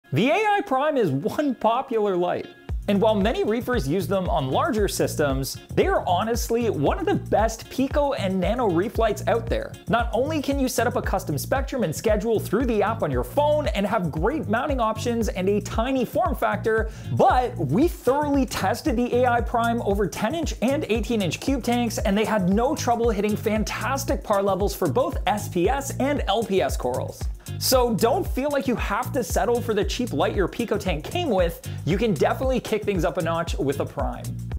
Is This the Best Nano Reef Tank Lighting Solution?
Nano and pico reef tanks and saltwater aquariums need a great light too, and AquaIllumination has you covered!